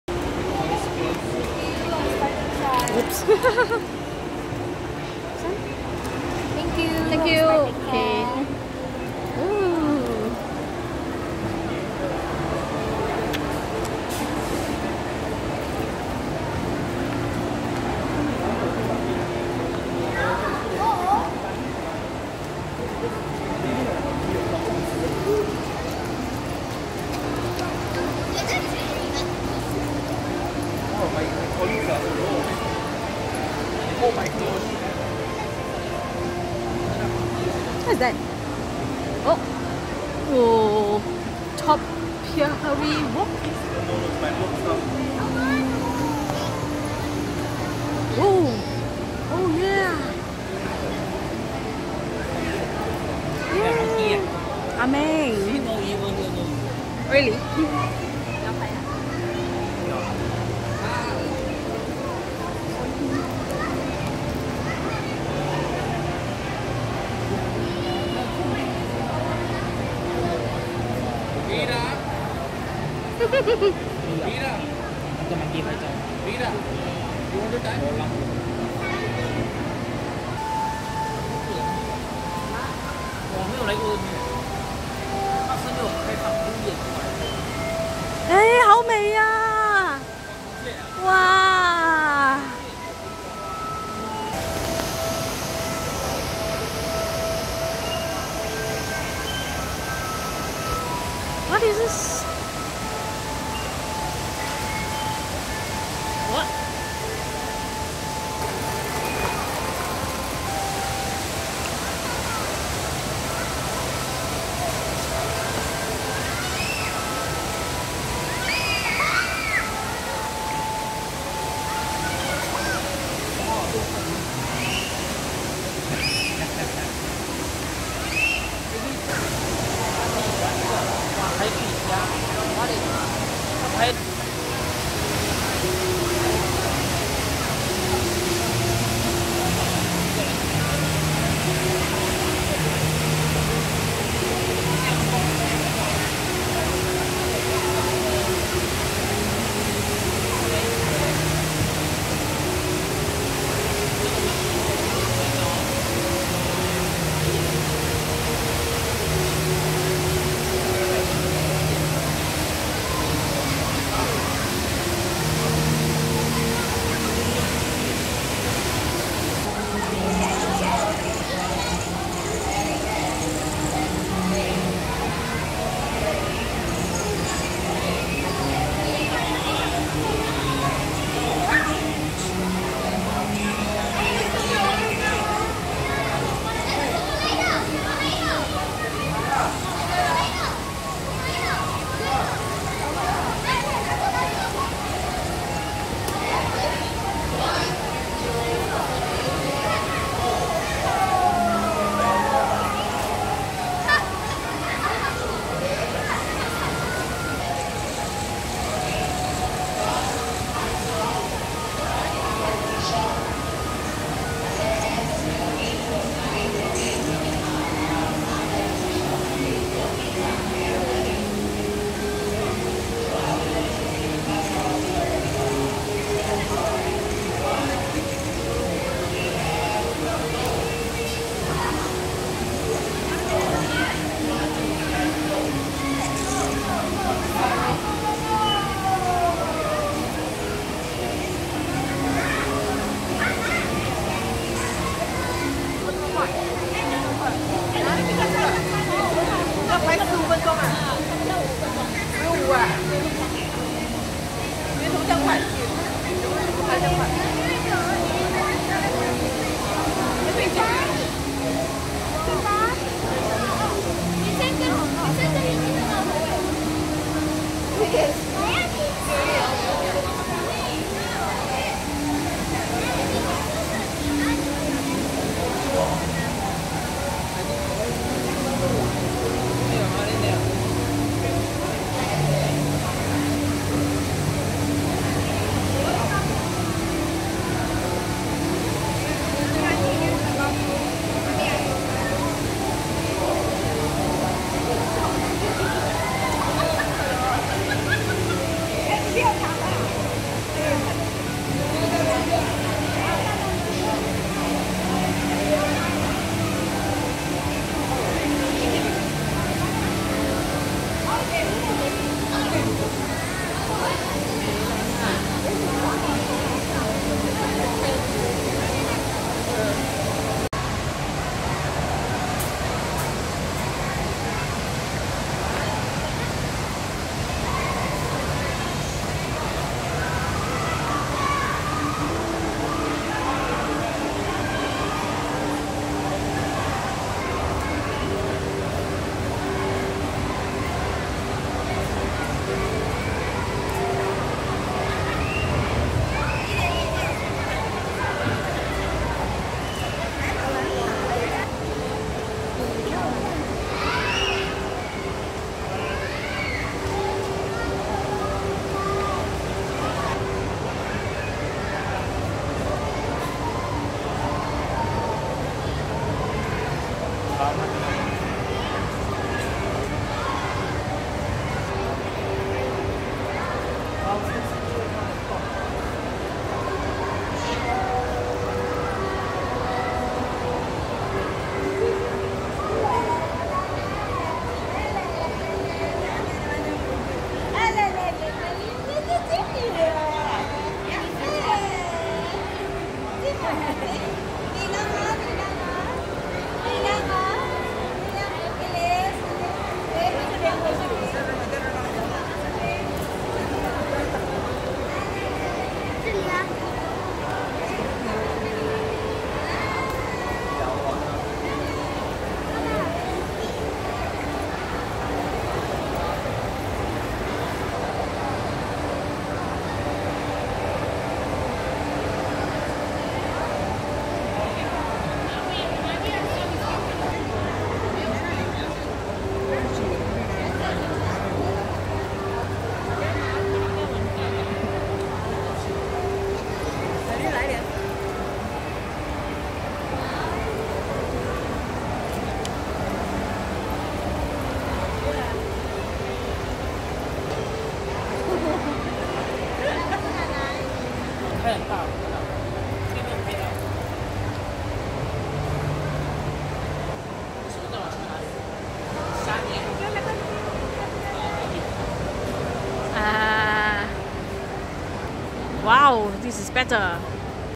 CANOPY PARK at JEWEL CHANGI AIRPORT SINGAPORE Full Tour
I tried everything at Jewel's new CANOPY PARK! This is at the top level of the Jewel and there's a few slides, giant hanging nets and mazes around this beautiful garden. Attractions featured: HSBC Rain Vortex, Topiary Walk, Discovery Slides, Manulife Sky Nets - Bouncing, Manulife Sky Nets - Walking, Hedge Maze, Mirror Maze, Canopy Bridge.